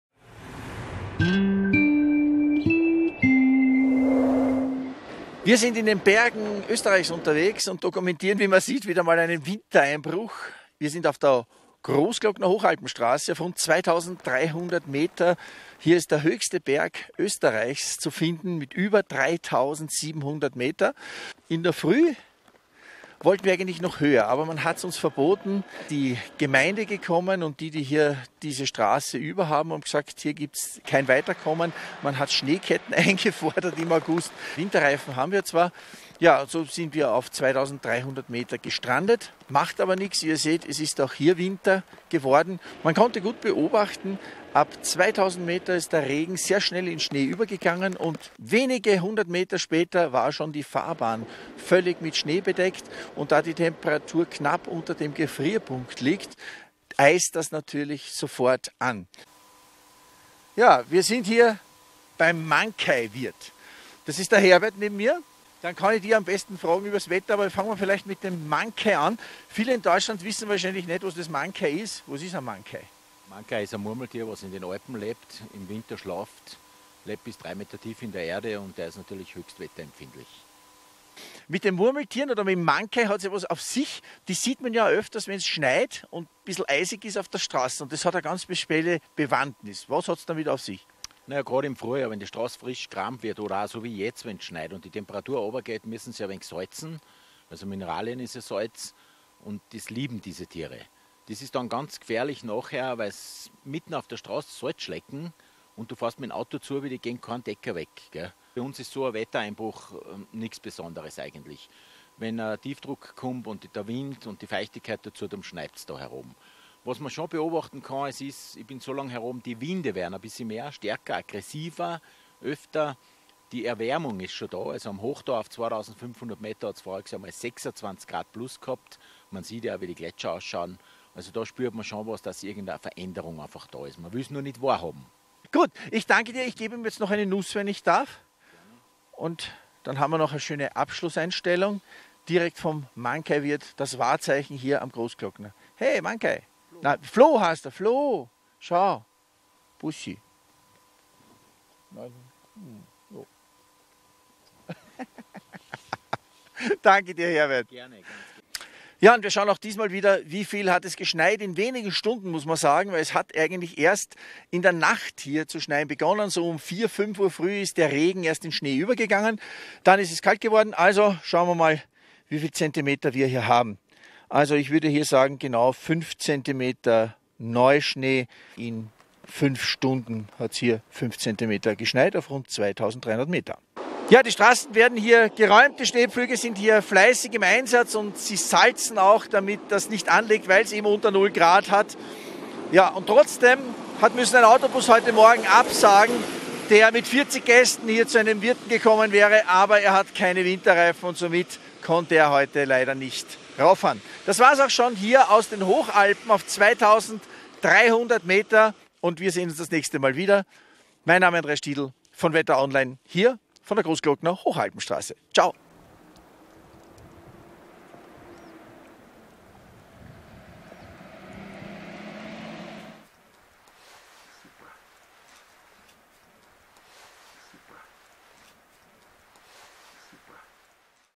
wetteronline.de: WetterReporter mitten im Augustschnee (10.8.2016)
Mitten im August hat der Winter in den Alpen am Mittwoch ein Gastspiel gegeben. Unser WetterReporter Andreas Stidl war auf der Großglockner Hochalpenstraße in rund 2300 Meter Höhe unterwegs gewesen, wo sich eine dünne Schneedecke bildete. Eigentlich wollte er aber noch höher hinaus...